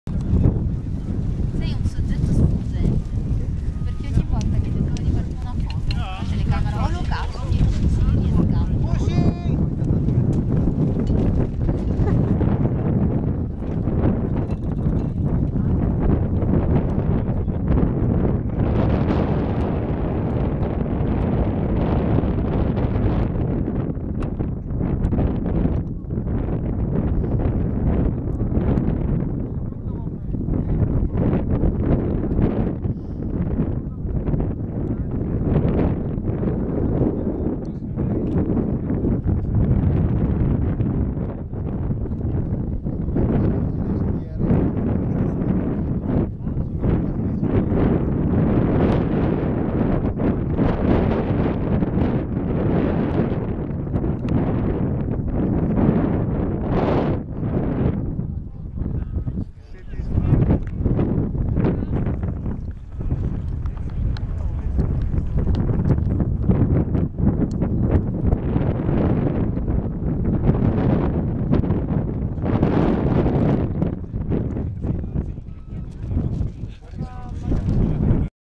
Despegue Daniel.MOV
Dia 1 de Competencia. Campeonato Mundial de Ala Delta Montecucco 2011.